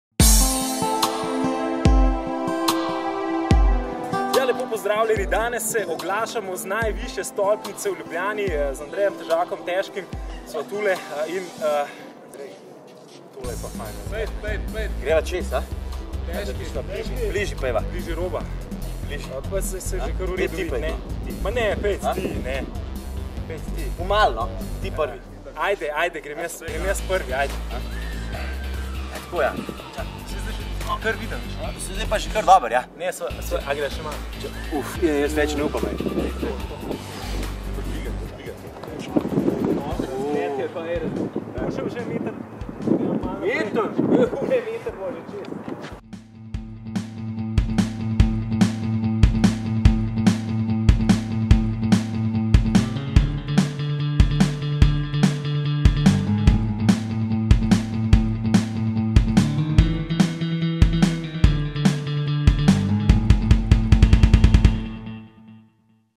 Alive Step Up, na strehi Kristalne palace
Kristalna Palača z vrha. Naša ambasadorja sta preverila kakšen je razgled z najvišje zgradbe v Sloveniji. Uuuuu piha, piha.